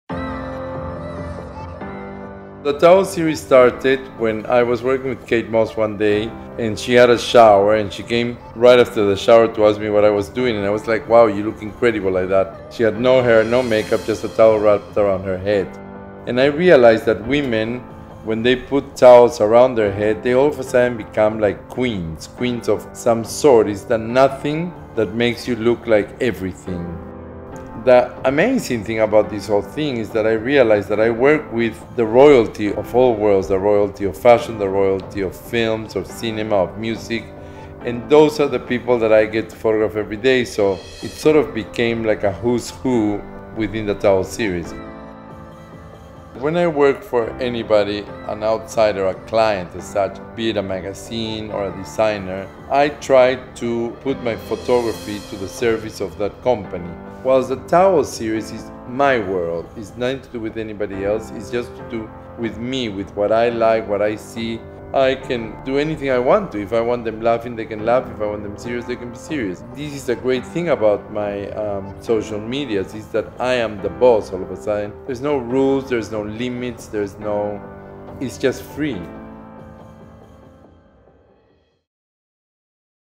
Bella Hadid: Under The Towel
We have a peek under the towel and behind-the-scenes in this exclusive film of Bella Hadid’s Towel Series shoot. The 19 year-old GQ Model of 2016 shows us how it’s done…